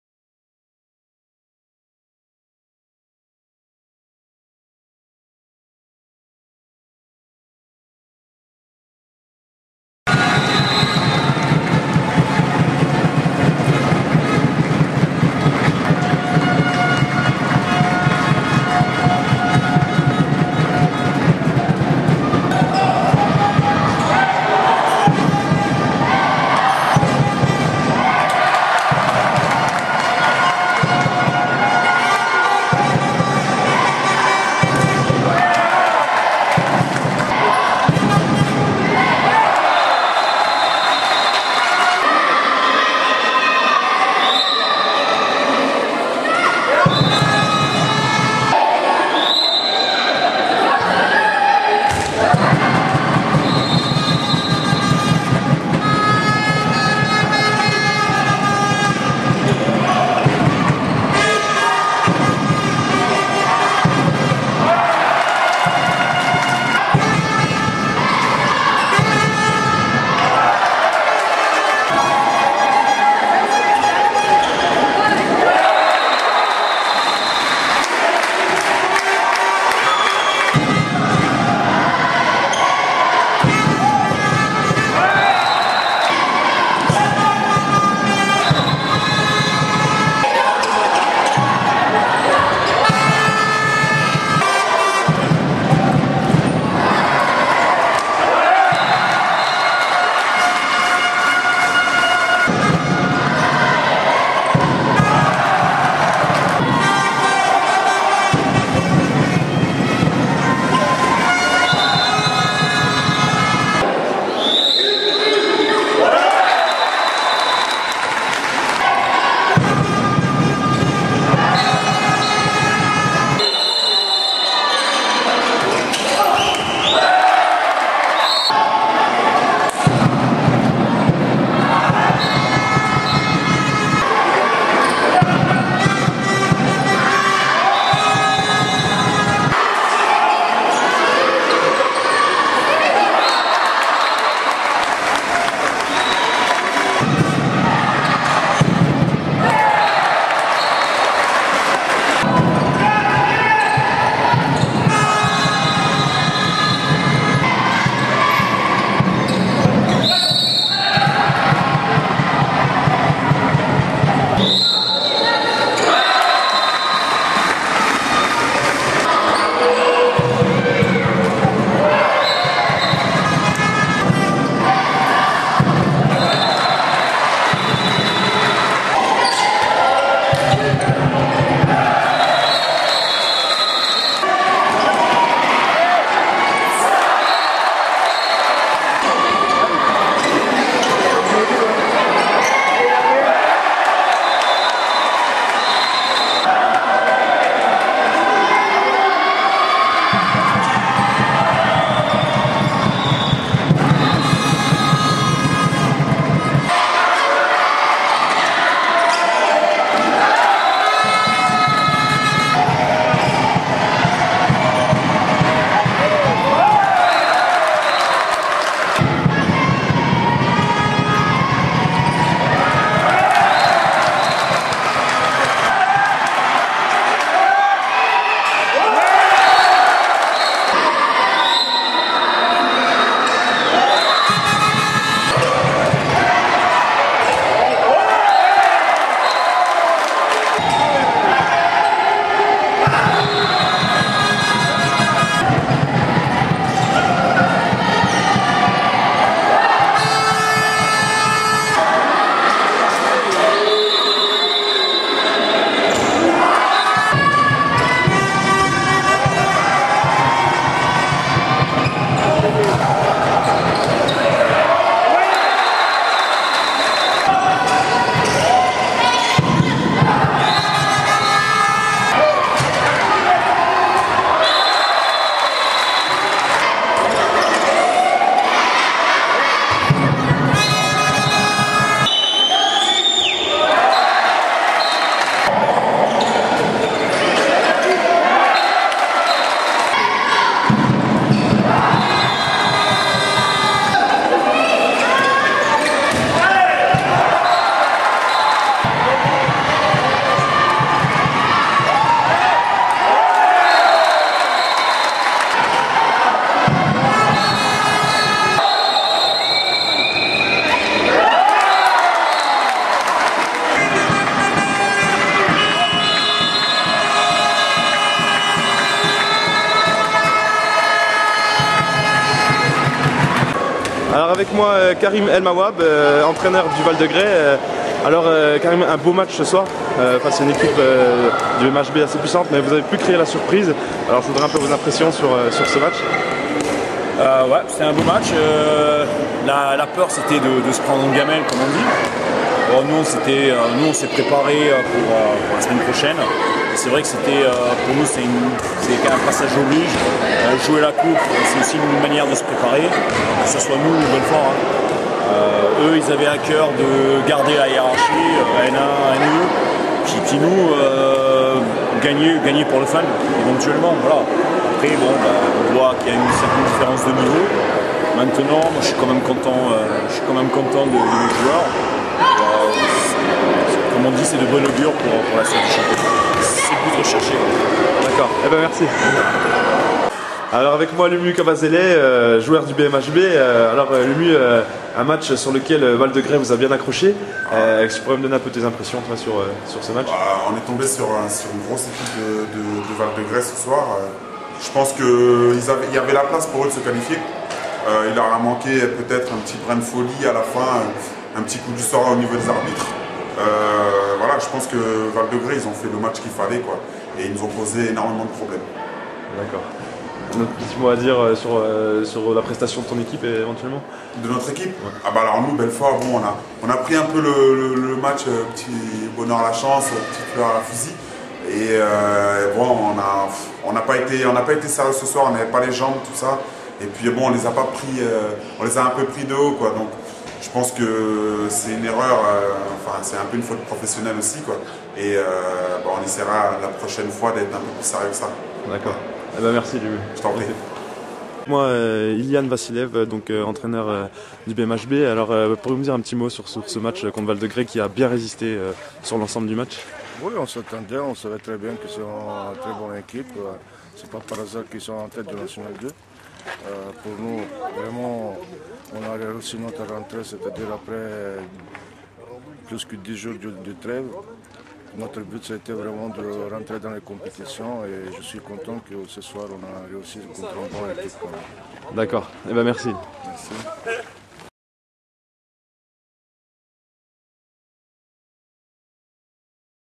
CDF HANDBALL : VAL DE GRAY (N2) - BMHB (N1) - lesinfosdusport.fr
Match comptant pour le troisième tour de la coupe de France - Val de gray 23 - 27 BMHB - résumé et interview